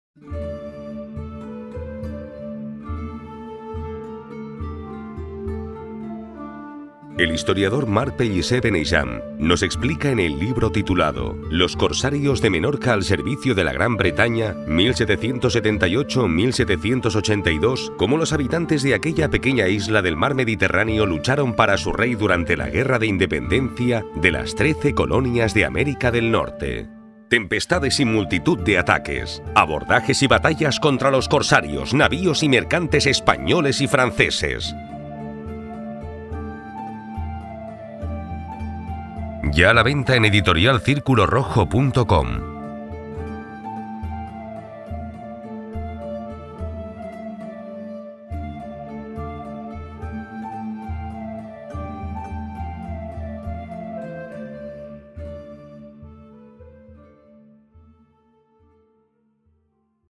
Los corsarios de Menorca al servicio de la Gran Bretaña (1778-1782) - Booktrailer
En el año 1775 estalló la mayor de las insurrecciones coloniales de la historia contra un imperio, la de las trece colonias británicas de América del Norte. La isla de Menorca, en el centro del Mediterráneo Occidental y también súbdita de la Gran Bretaña desde 1713, poco tendría que ver con un conflicto que se estaba dirimiendo en el otro extremo del Atlántico. No obstante, los avatares de la guerra implicaron a los menorquines cuando Francia y España declararon la guerra a los británicos el 1778.
Un conflicto lejano pasó a dirimirse en el mismísimo mar Mediterráneo y Menorca, junto con Gibraltar, se convirtieron en los pivotes británicos de aquellas pequeñas y hostiles aguas. Exentos por derecho foral de cualquier tipo de servicio militar, los menorquines lucharon embarcados en multitud de pequeños buques armados en corso. Es precisamente en este libro donde se nos darán a conocer las apasionantes historias de aquellos intrépidos corsarios de Menorca en un mar infestado de enemigos a batir.